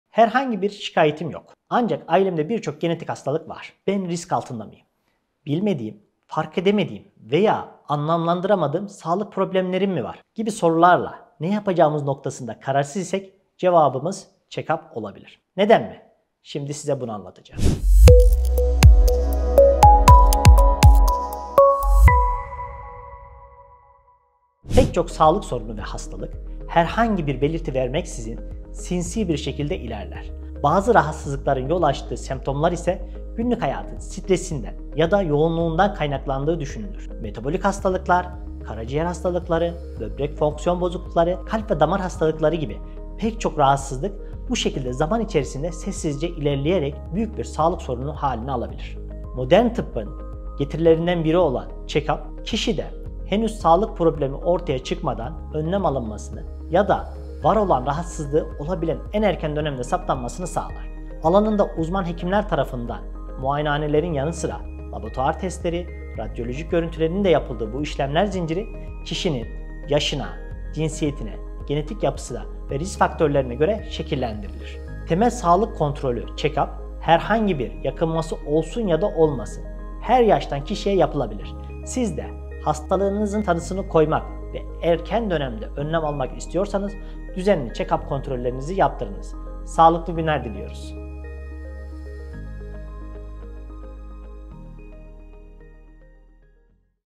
Sağlıkta Erken Teşhis: Check-Up
Check-up işlemi hakkında merak ettiğiniz sorulara Uzm. Dr. Selahattin Erdem’in anlatımıyla cevap bulabilir, randevu almak için hastanemize başvurabilirsiniz. 🧡